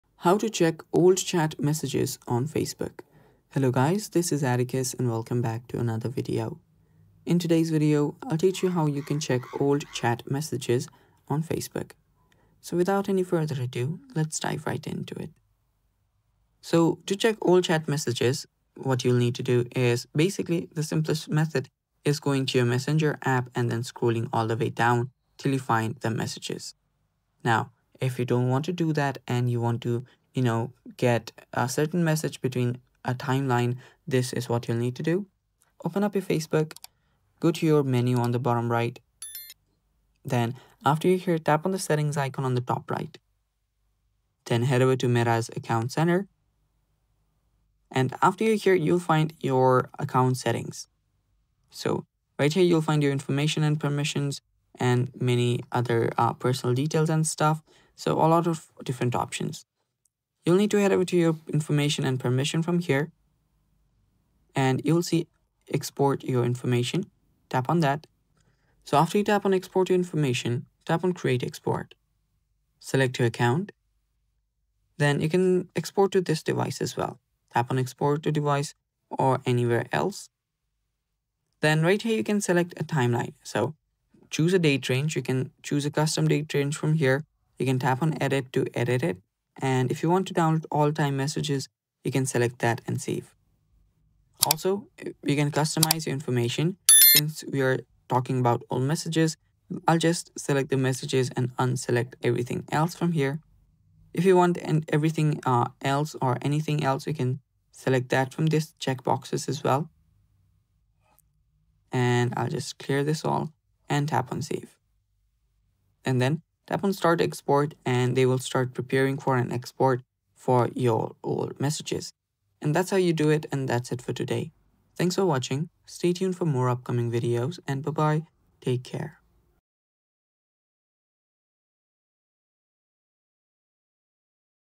How To Check Old Chat Messages On Facebook
Want to find that old conversation on Facebook but don’t know where to start? 💬🔍 This quick and easy 2025 guide will show you how to check old chat messages on Facebook — whether you’re using the app, Messenger, or desktop version!

What You’ll Learn in This Video:
✅ How to search old chat messages on Facebook Messenger (2025 update)
✅ How to find archived and hidden conversations
✅ Tips to quickly navigate long chat histories
✅ How to recover old messages you thought were gone

Whether you’re trying to revisit past memories or locate important info, this tutorial will help you access your old messages effortlessly.

📌 Chapters:
⏱ 0:00 - Introduction
⏱ 0:10 - How To Find Old Chats on Facebook Messenger